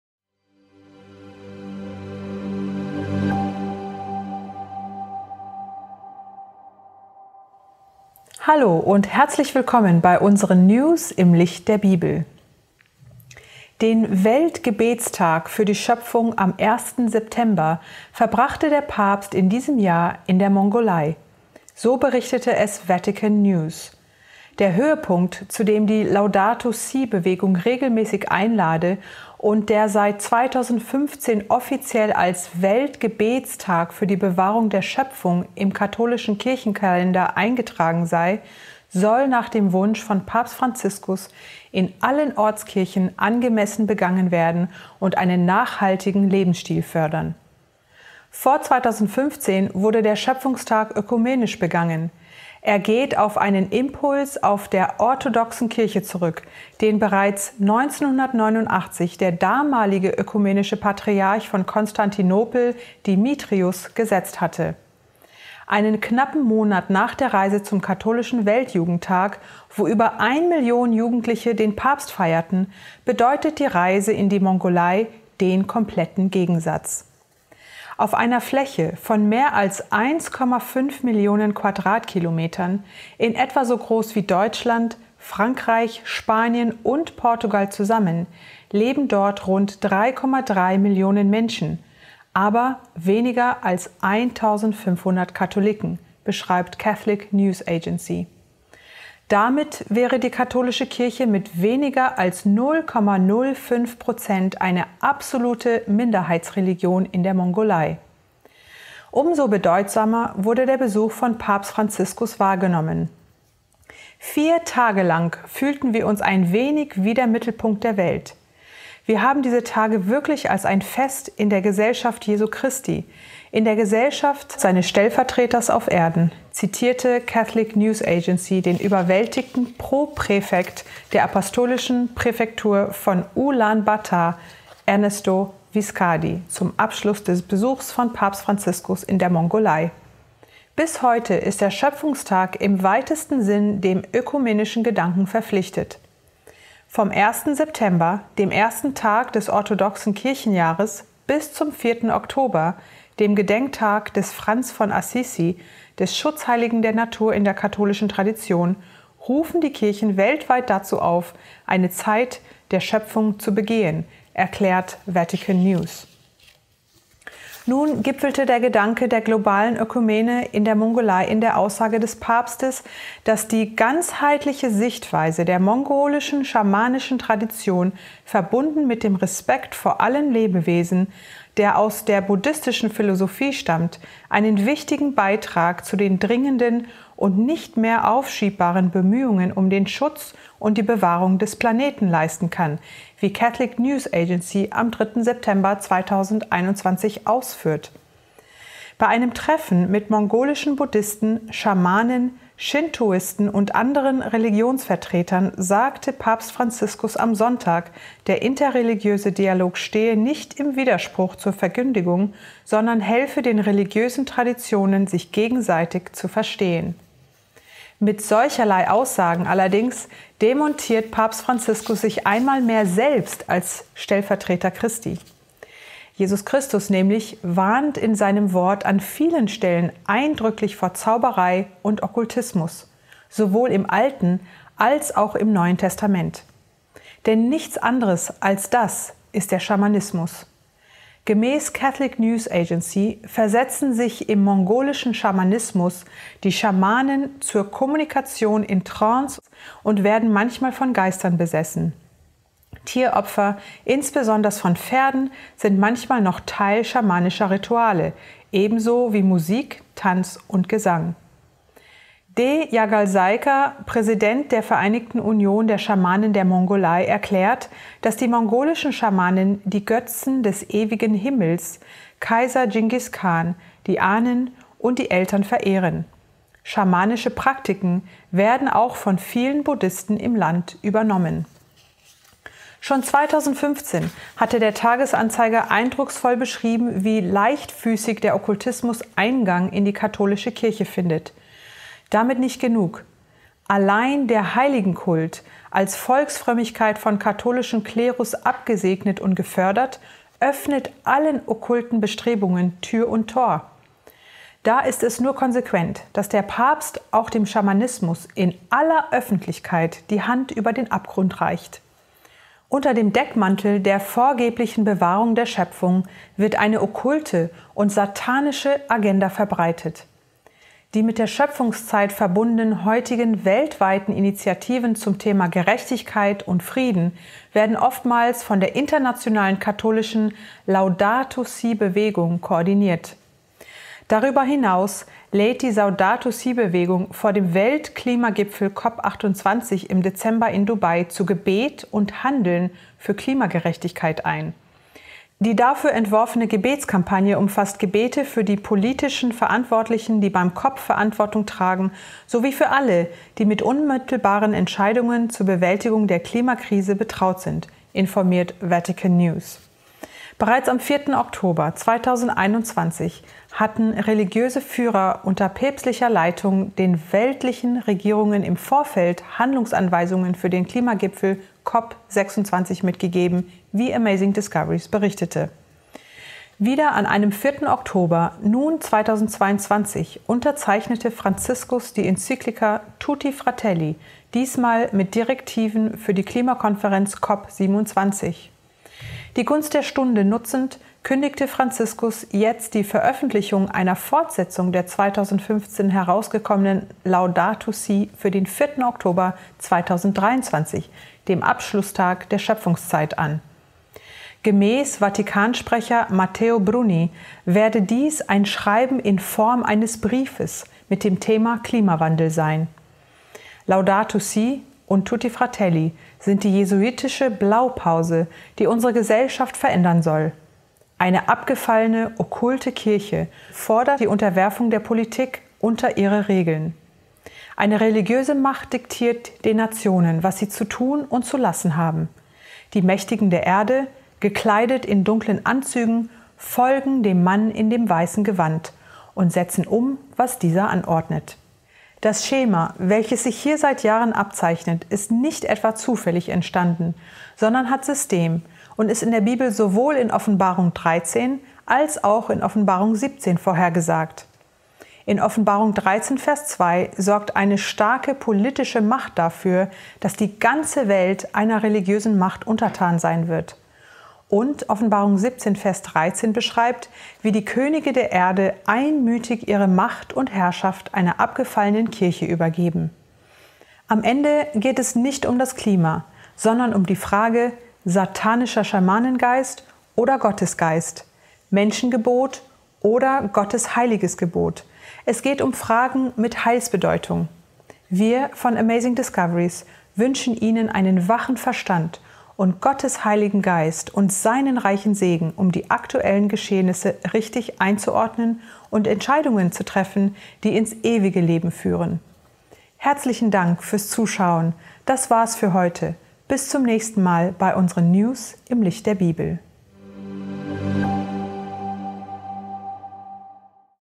News: Mongolei: Papst reicht Schamanismus die Hand - Laudato Si Teil II angekündigt
Mongolei: Papst reicht Schamanismus die Hand - Laudato Si Teil II angekündigt

Den Weltgebetstag für die Schöpfung am 1. September verbrachte der Papst in diesem Jahr in der Mongolei. So berichtete es Vatican News. Der Höhepunkt, zu dem die Laudato Si’-Bewegung regelmäßig einlade und der seit 2015 offiziell als „Weltgebetstag für die Bewahrung der Schöpfung“ im katholischen Kirchenkalender eingetragen sei, soll nach dem Wunsch von Papst Franziskus „in allen Ortskirchen angemessen begangen werden und einen nachhaltigen Lebensstil fördern. Vor 2015 wurde der „Schöpfungstag“ ökumenisch begangen; er geht auf einen Impuls aus der orthodoxen Kirche zurück, „den bereits 1989 der damalige Ökumenische Patriarch von Konstantinopel, Dimitrios, gesetzt hatte.“